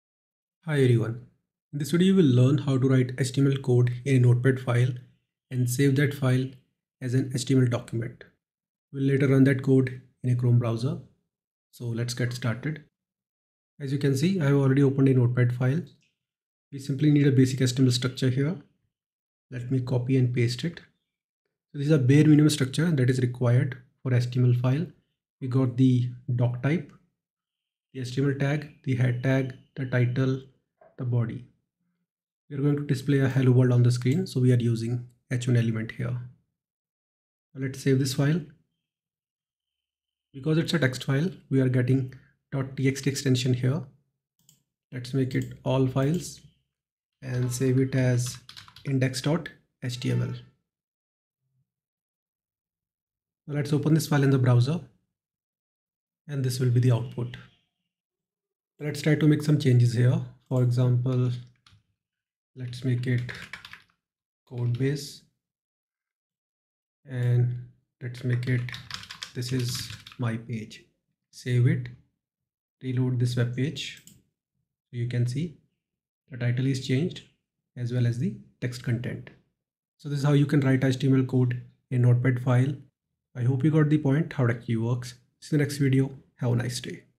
How to Run HTML Program using Notepad and Chrome
In this video, you will learn how to run html program using notepad and chrome. I hope you'll like the video and leave your feedback and suggestions for future video tutorials.

0:00 Intro
0:14 Write HTML Code in Notepad
0:37 Save Notepad File as HTML
0:54 Final Output